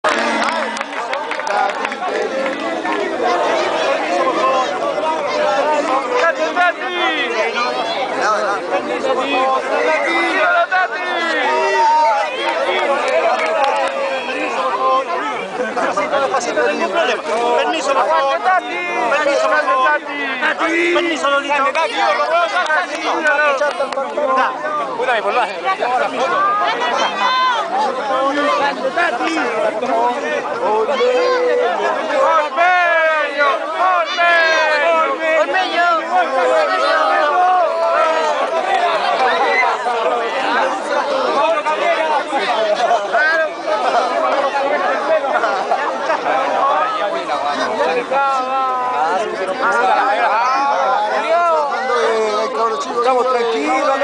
Salida del tati & ormeño del sta laura / 8/8/2010
salida del tati y de ormeño del estadio santa laura
8 de agosto 2010
palestino 0
ucatolica 1